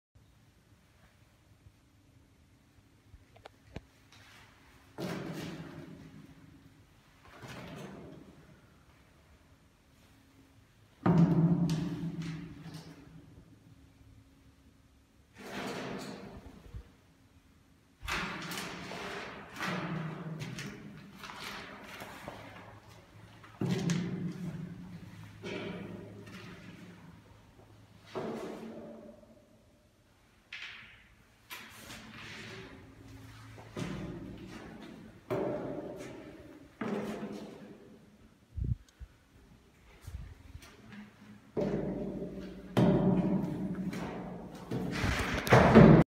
The Boatrooms - More Recovered Footage
Some more recordings recovered.

There is currently no information about this location so far, but some note it to be the inside of a boat/hull, that seems to go infinitely.